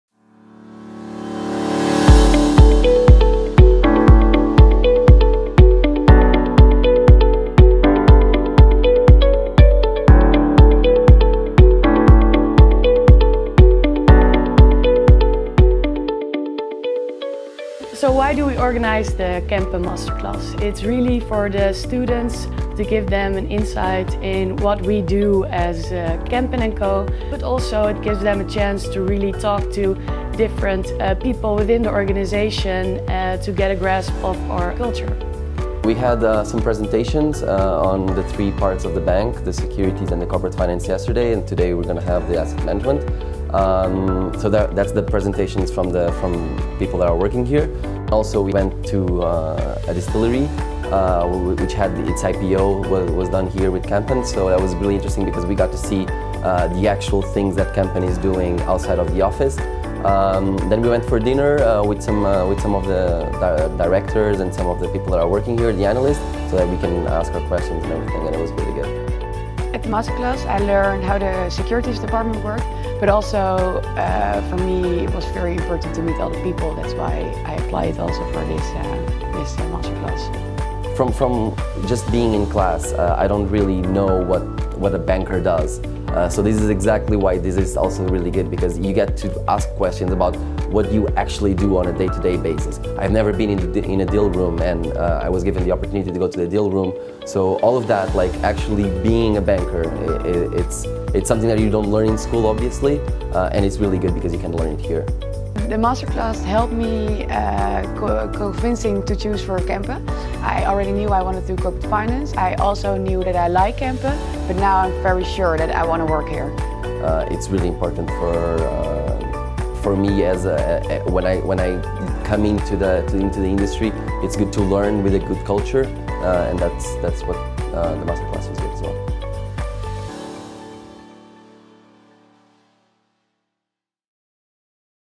Aftermovie Kempen Masterclass 2017
The Kempen & Co Masterclass 2017 after movie is live! Many thanks to all the students and colleagues who made these two wonderful days happen. In these short 48 hours we were amazed to see the amount of future high potential candidates. Please enjoy and get inspired.